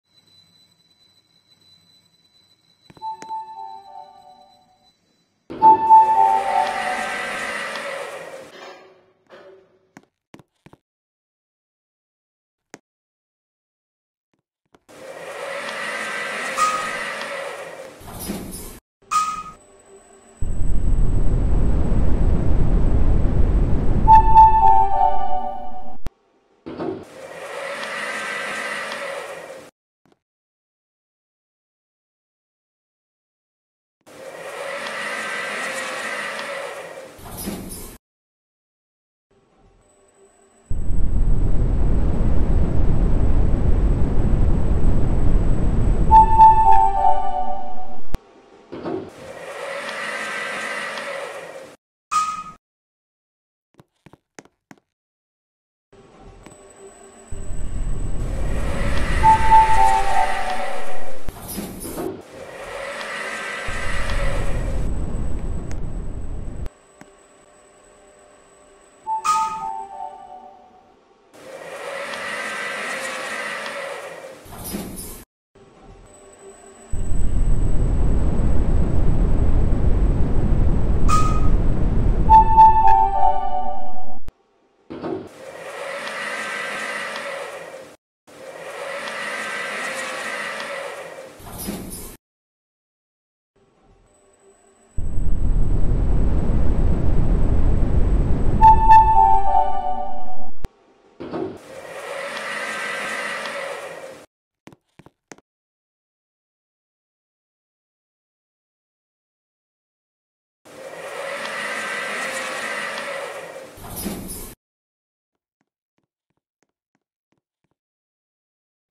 July 15, 2023: 2x Hyundai Traction Parking Elevators @ Hyundai Test Tower - Seoul, SKR!!!
Credit to

Floors served: B3-B1, G, M

Floor B3 is blocked off because it’s probably for staff only or maybe it’s under construction/renovation!!!